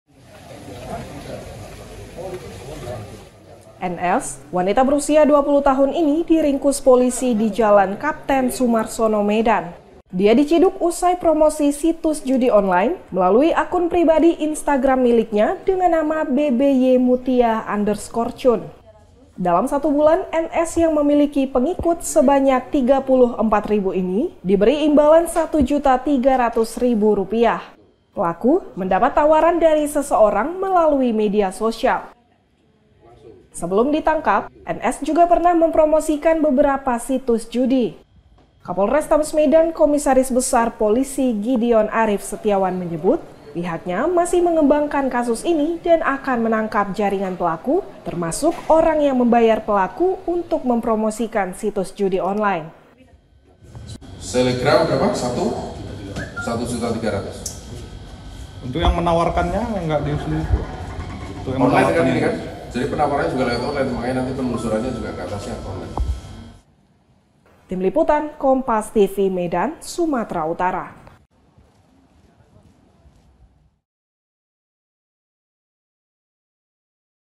Polrestabes Medan Tangkap Selebgram yang Promosi Situs Judi Online
MEDAN, KOMPAS.TV - Seorang selebgram ditangkap petugas Satreskrim Polrestabes Medan.

Wanita muda ini diduga turut mempromosikan situs judi online lewat akun instagramnya.

Pelaku diberi imbalan Rp 1,3 juta. (*)